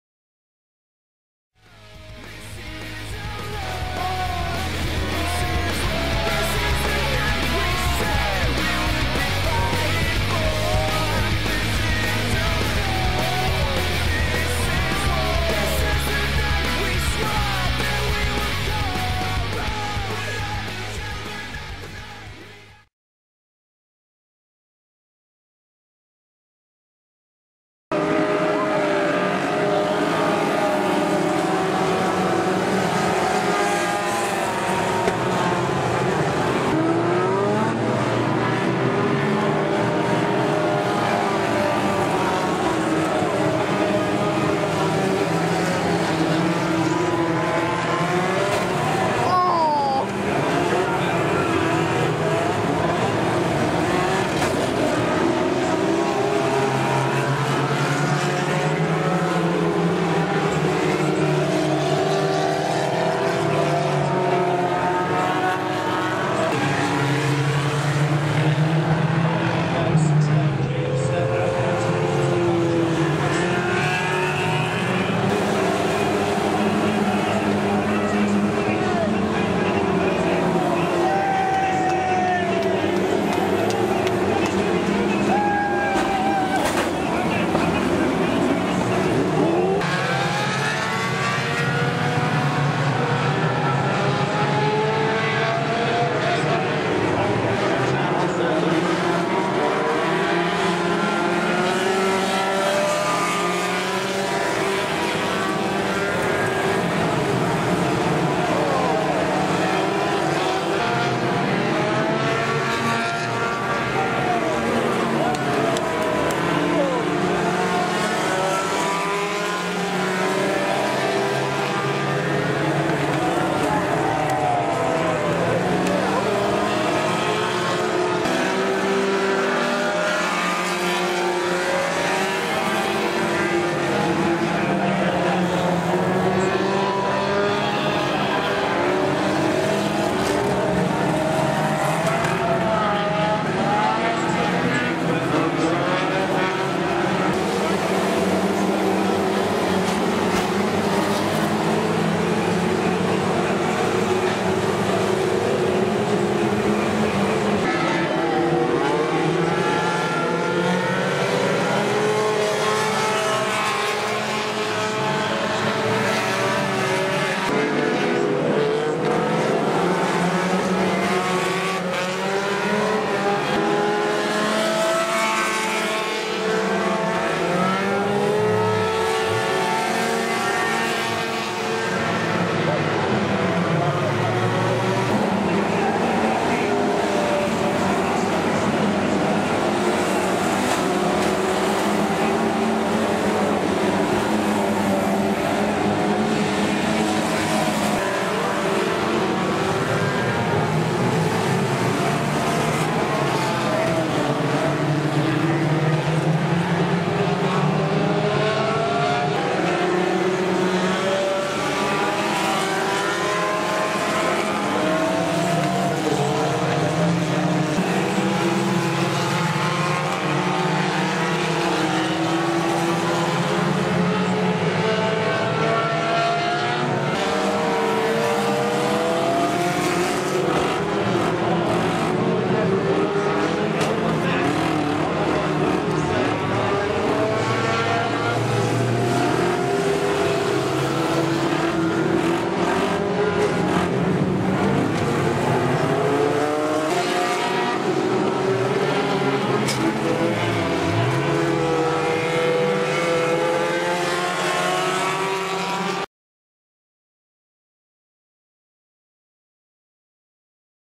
Kings Lynn 1500cc teams 2011
There was some huge hits and good action all around for the 1500cc teams, best wishes go to the driver who suffered major injuries from a huge T-bone (not included)